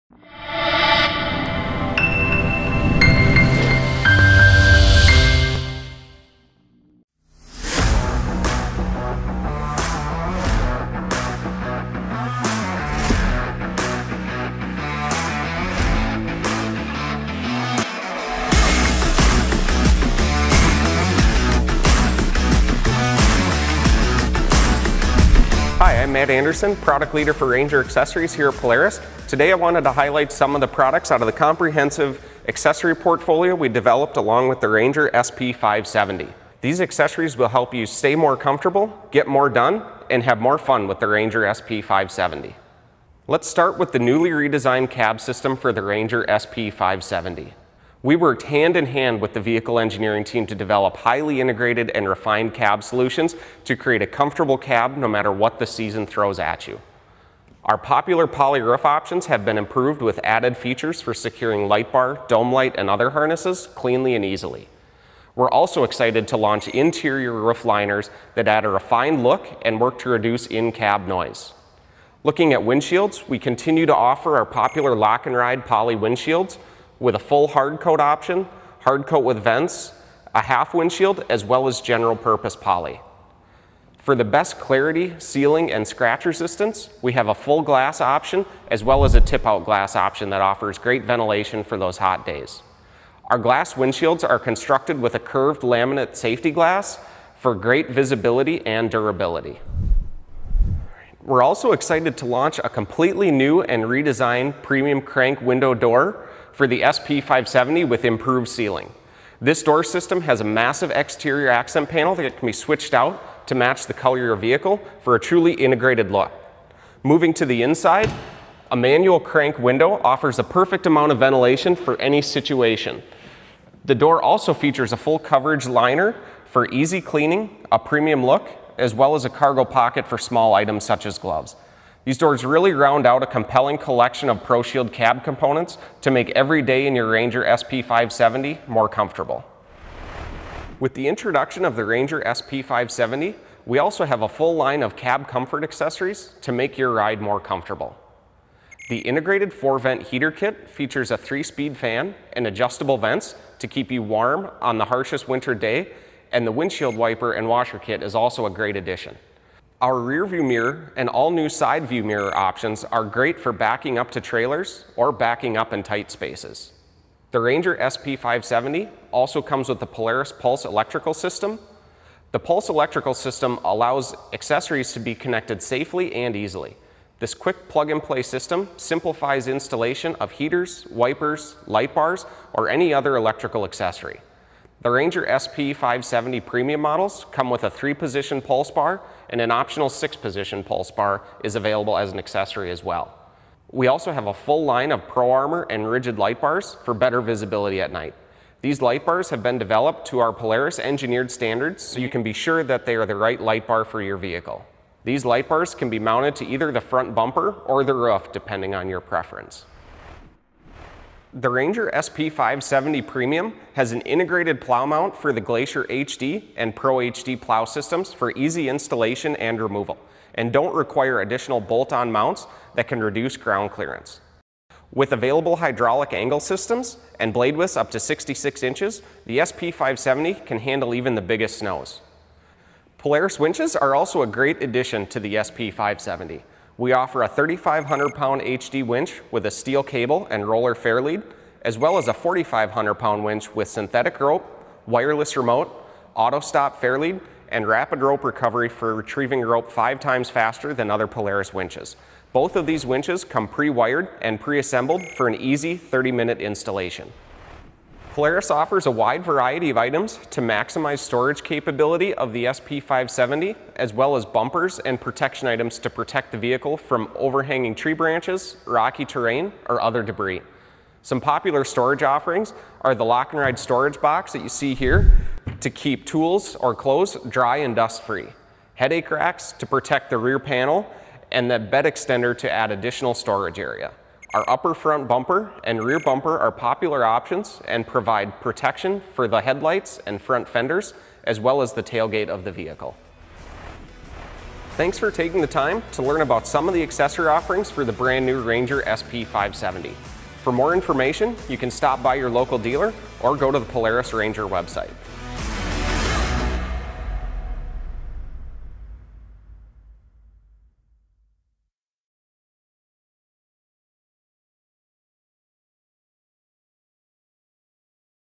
2022 Ranger SP 570 ACCESSORY Walkaround
Accessories, accessories, and more accessories on the all-new Polaris Ranger SP 570.

Whether it's a Pro Shield cab system for year-round comfort, Pro HD winches for adding pulling power, or Lock & Ride storage to keep gear or supplies secure, this accessory lineup offers limitless ways to customize to fit your riding style.